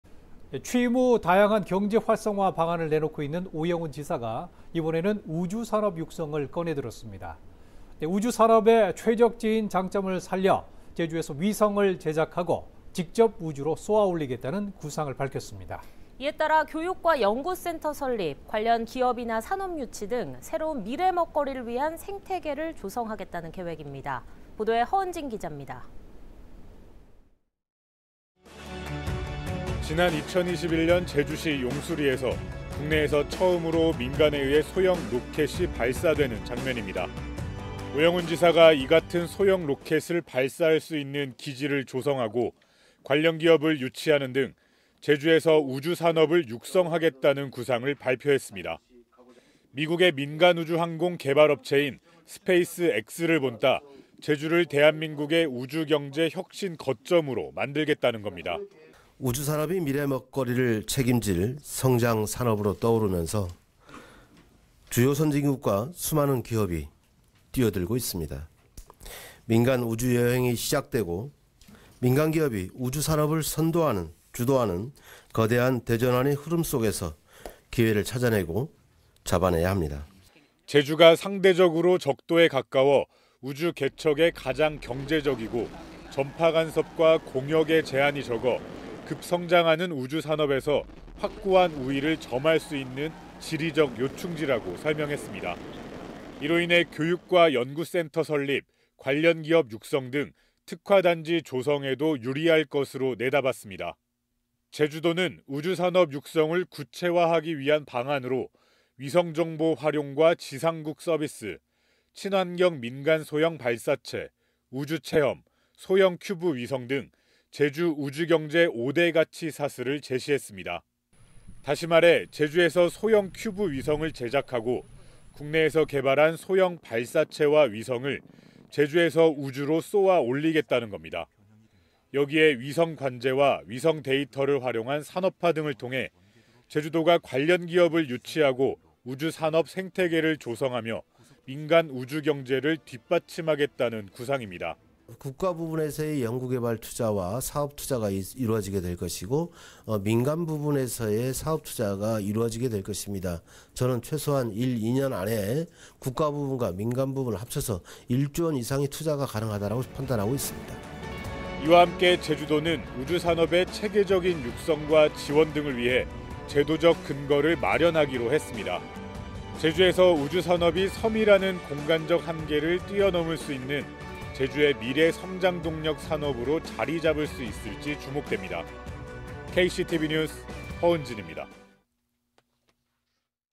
'섬을 넘어 우주로'…제주 우주산업 육성
오영훈 지사가 이번에는 우주산업 육성을 꺼내들었습니다.

제주에서 위성을 제작하고 직접 우주로 쏘아 올리겠다는 구상입니다.

이로 인해 교육과 연구센터 설립, 관련 기업이나 산업 유치와 육성 등 새로운 미래 먹거리를 위한 생태계를 조성하겠다는 구상입니다.